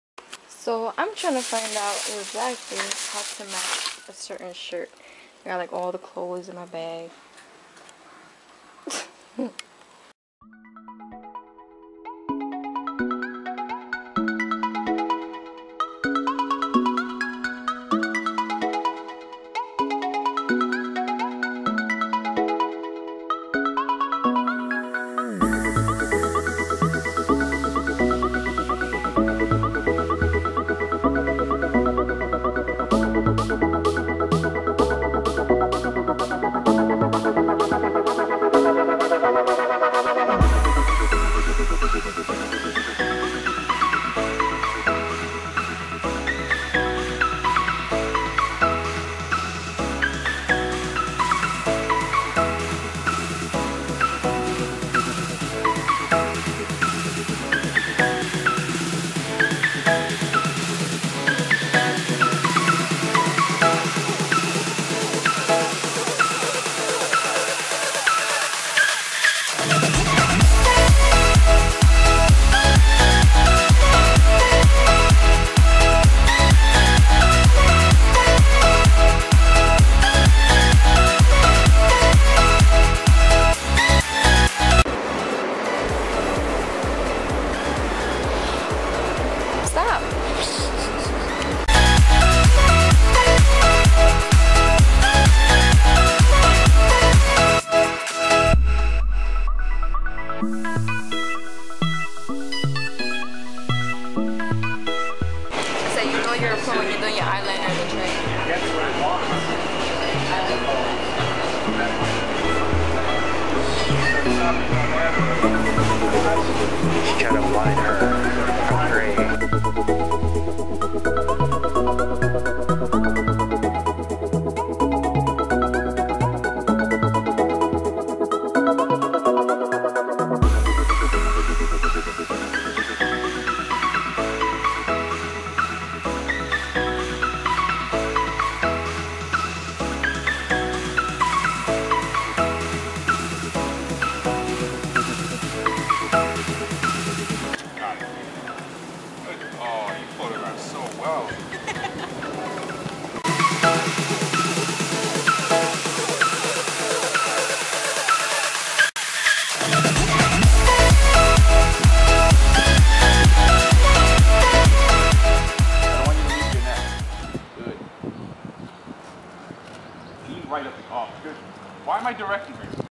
Train Makeup Tutorial & Photoshoot [MetalMakeupxo]
Hey guys
Sorry for the delay in videos, I literally couldnt find my camera!
But I'm back!

Photographer Harts Ortiz
Instragram @HARTSORTIZ_PHOTOGRAPHER
youtu.be/9WCWTTzU6pM

Facebook.com/metalmakeup32610